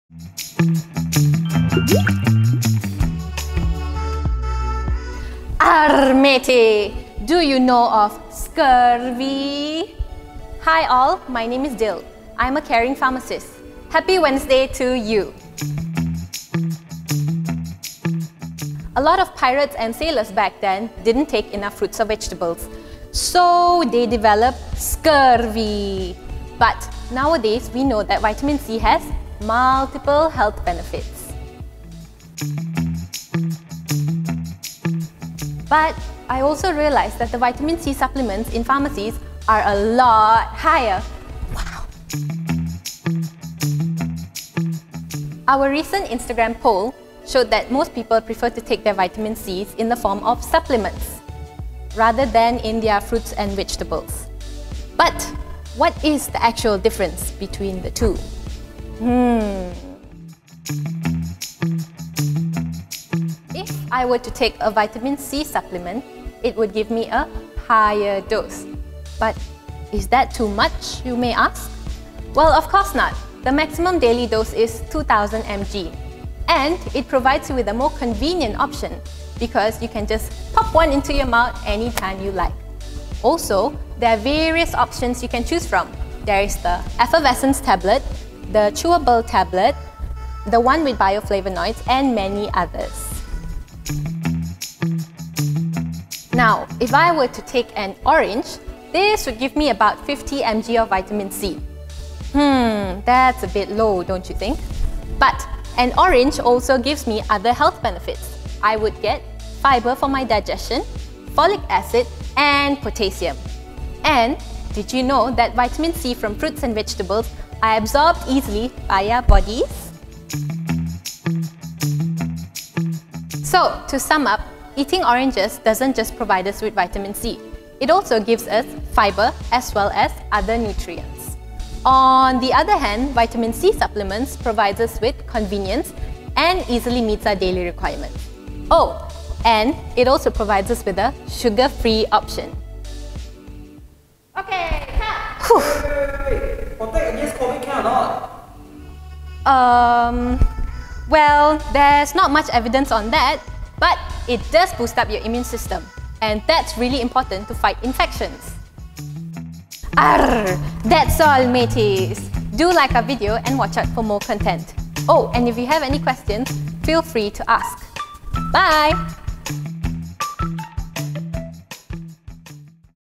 [Wellness Wednesday] EP 1:🍊Oranges or Vitamin C supplements
🍊Oranges or Vitamin C supplements...which one do you think has a greater benefit for your health? 🤔

Watch this video where Dilhara will be sharing some myths & facts about this topic!

0:00 Introduction
0:17 Did you know that Vitamin C is good for our gums?
0:34 Did you know a normal Malaysian adult requires only 70mg a day of Vitamin C